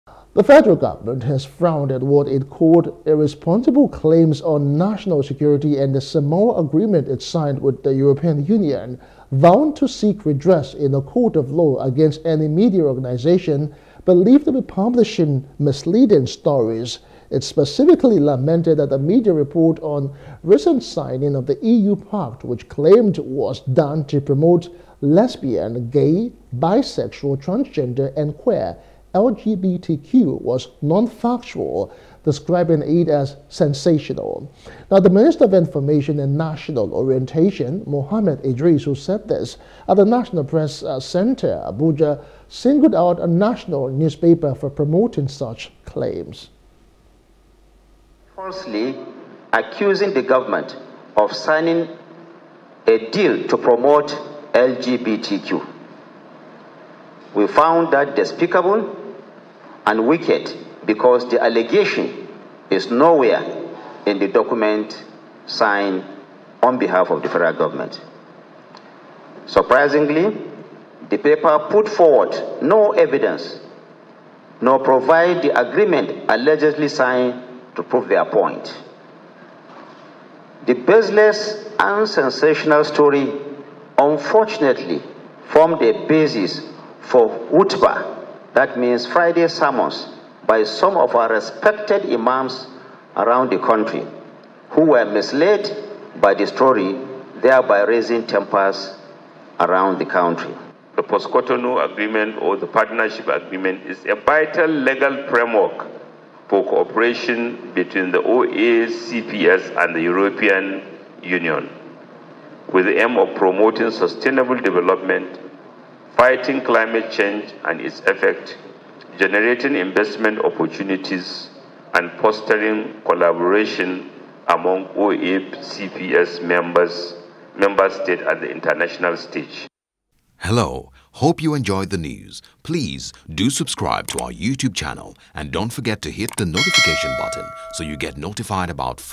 Samoa Agreement: No Clause On Lgbt, $150b For Nigeria – Bagudu
The Federal Government, yesterday, frowned at irresponsible claims on national security and the SAMOA Agreement it signed with the European Union, EU, vowing to seek redress in a court of law against any media organization believed to be publishing misleading stories. It specifically lamented that a media report on the recent signing of the EU pact, which claimed it was done to promote Lesbian, Gay, Bisexual, Transgender, and Queer, LGBTQ, was non-factual, describing it as sensational. Minister of Information and National Orientation, Mohammed Idris, who said this at the National Press Centre, Abuja, singled out a national newspaper, not Vanguard, for promoting such claims.